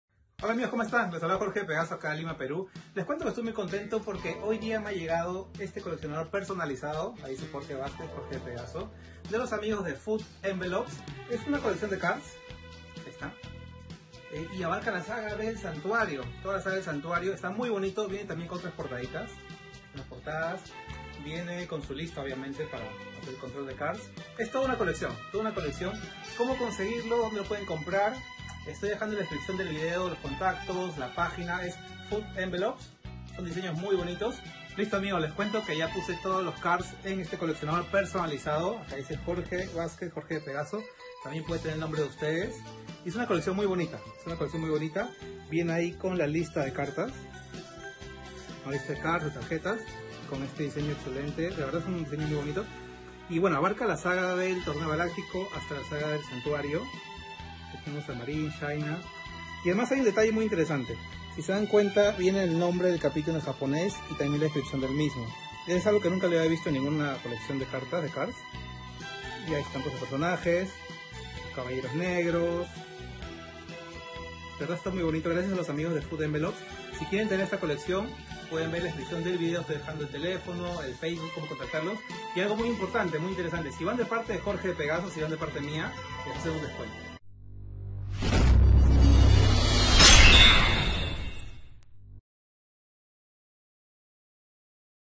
Cards de Saint Seiya por Fut Envelopes
Les recomiendo esta colección de cards de la saga del Torneo Galáctico y Santuario.
Es una colección de cards de cada capítulo que trae diseños muy buenos y coleccionador con portadas extras.
Pedidos al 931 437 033 o a Fut Envelopes en facebook 👈